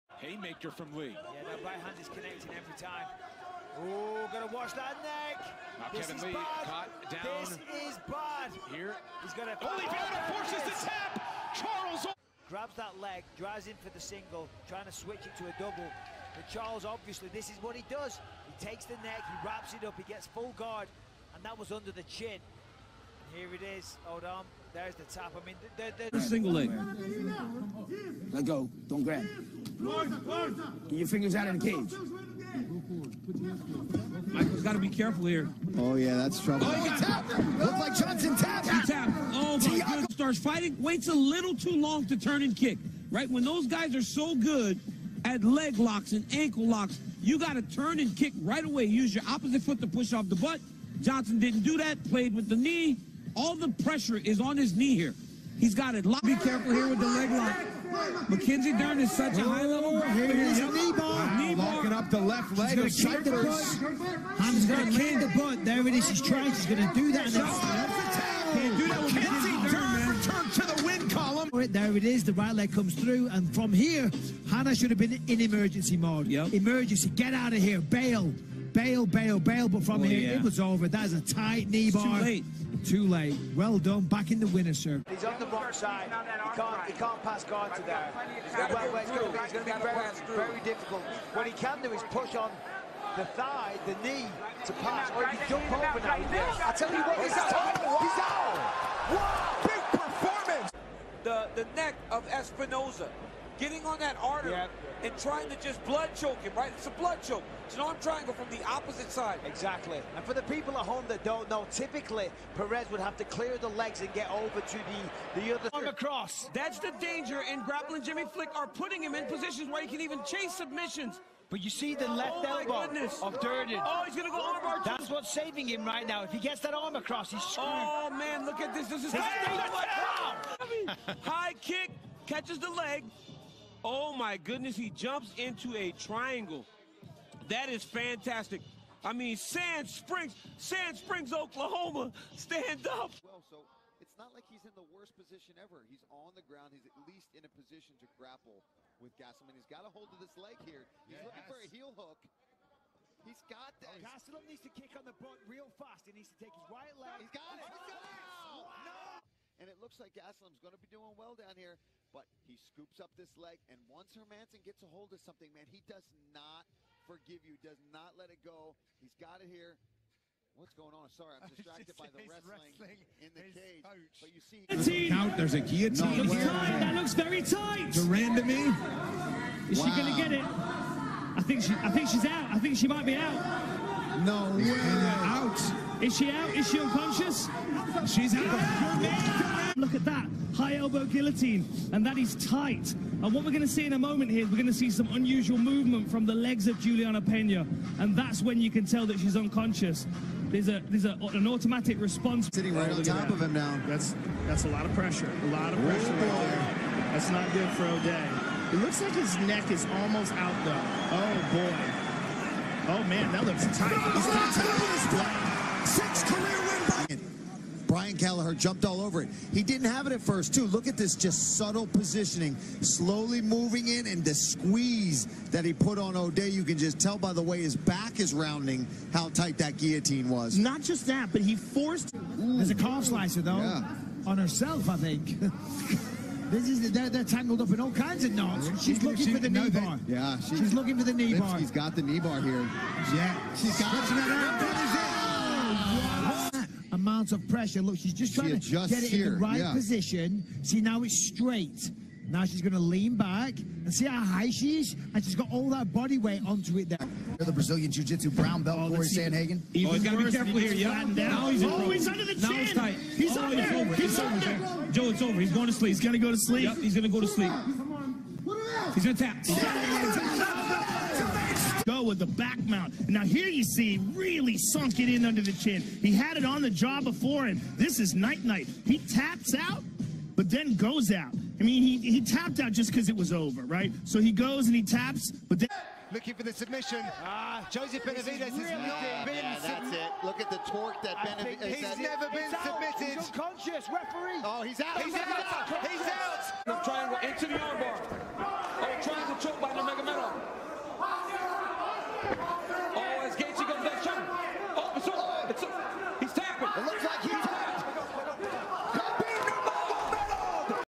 Best UFC Submissions 2020
These are the best UFC submissions of 2020 (in no particular order). Let us know which was your favourite below and if you think we missed any.